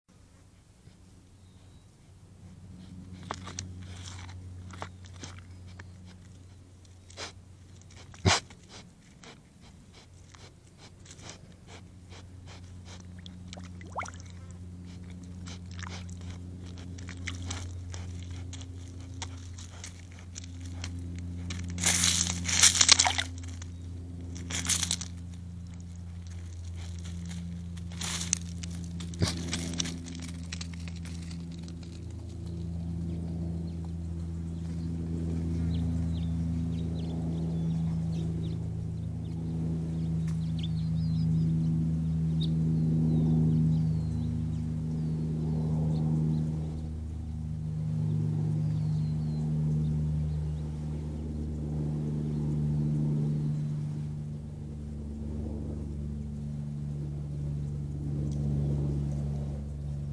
Javelina Snorts in Your Face, Kicks Rocks into the Watering Hole
Yep, that's what it did - it walks through the water, snorts right in your face, kicks rocks into the watering hole and walks off. What a bully!